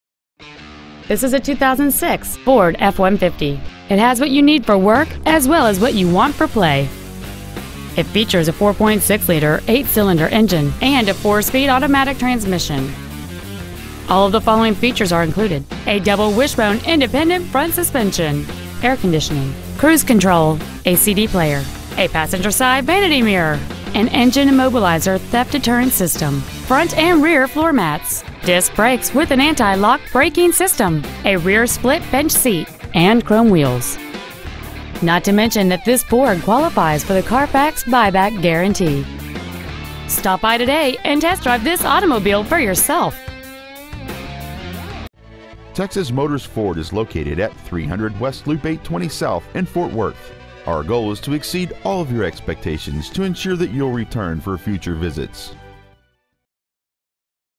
2006 Ford F-150 Arlington TX
Year : 2006
 Make : Ford
 Model : F-150
 Engine : Gas V8 4.6L/281
 Trans . : Automatic
 Exterior : Oxford White
 Miles : 134,384
 Interior : Tan

 300 West Loop 820 South

 Pre-Owned 2006 Ford F-150 Fort Worth TX